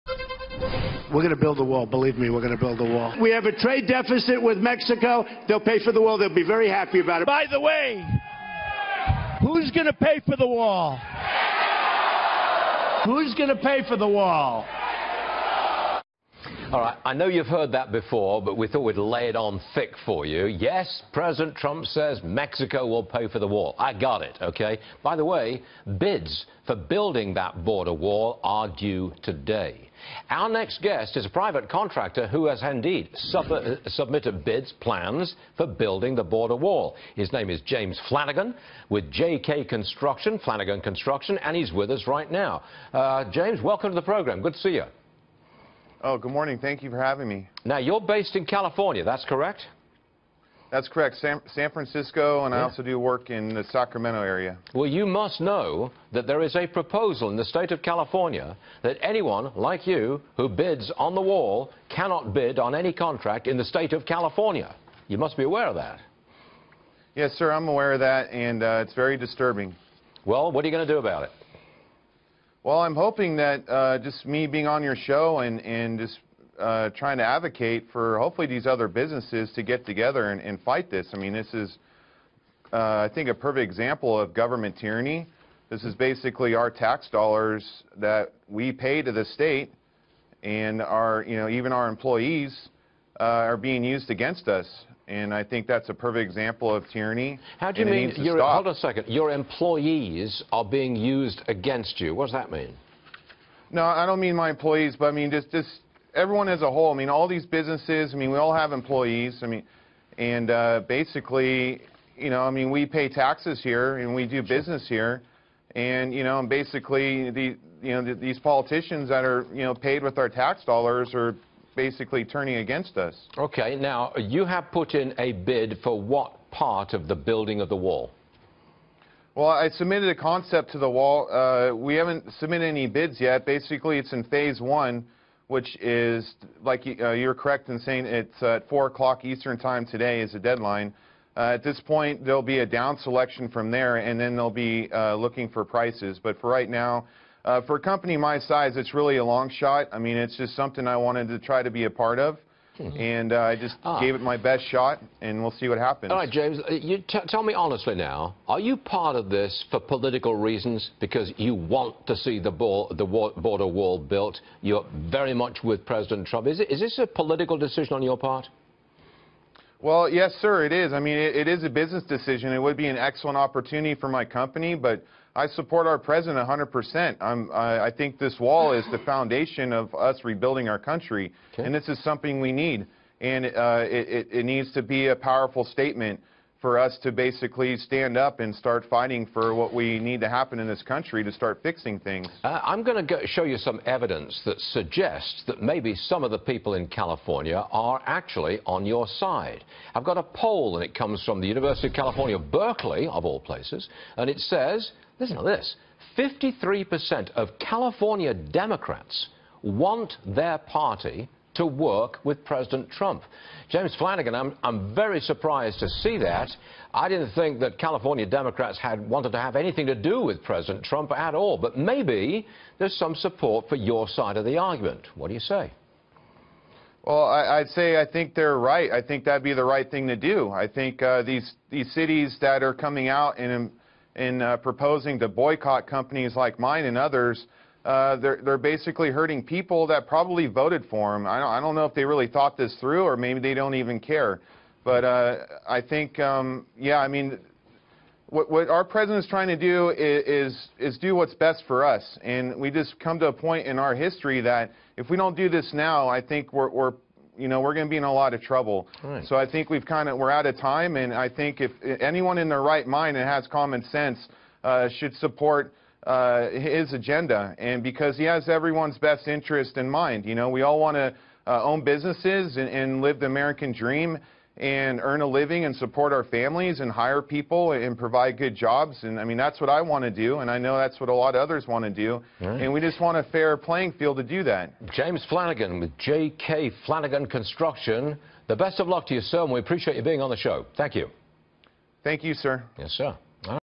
Why this construction owner can’t bid on Trump’s border wall
JK Flanagan Construction Owner James Flanagan on submitting a concept to President Trump’s border wall.